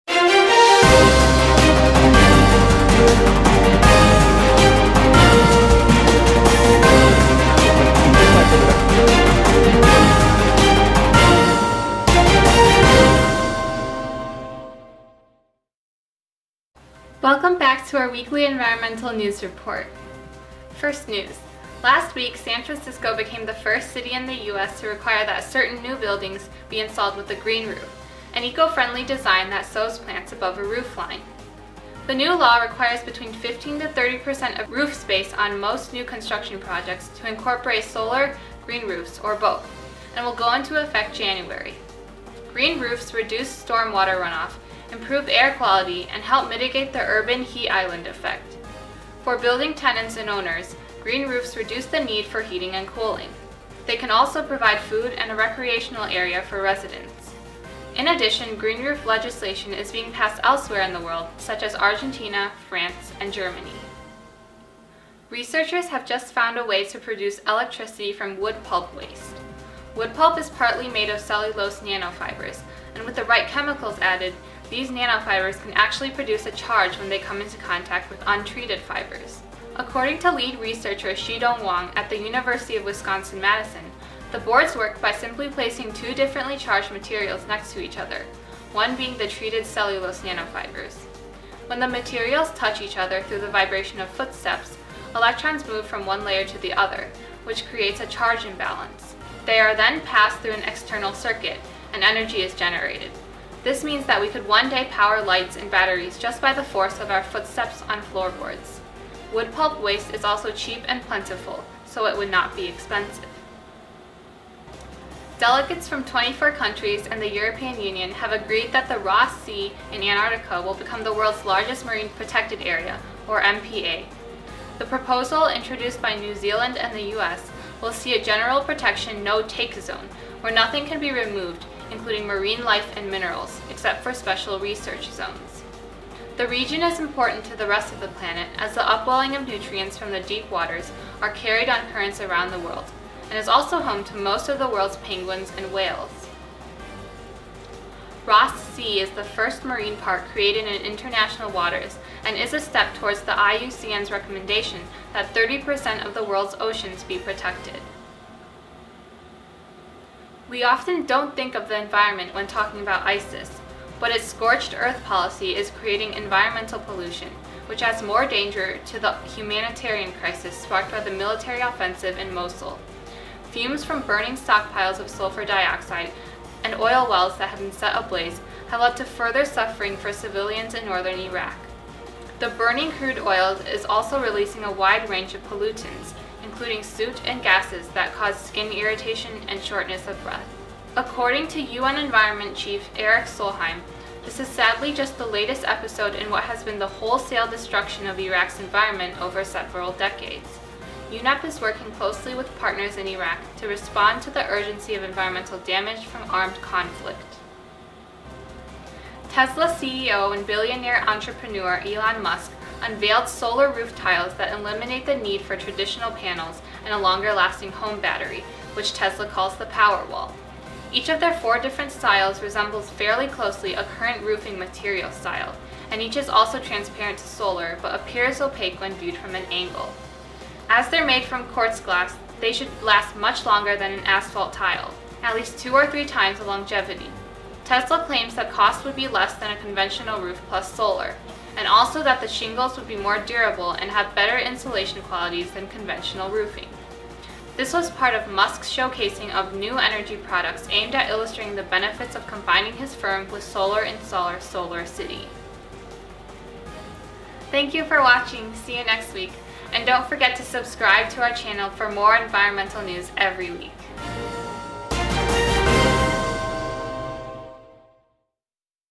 Weekly Environmental News Report | greenversal
This week's Environmental News Headlines (October 24 - 28, 2016):

1. San Francisco becomes the first city in the US to require that certain new buildings be installed with a green roof
2. UW Researchers find new source of renewable energy in wood pulp waste
3. World’s Largest Marine Reserve Created off Antarctica
4. ISIS “scorched earth policy” is creating environmental pollution
5. Tesla CEO Elon Musk Unveils Solar Roofs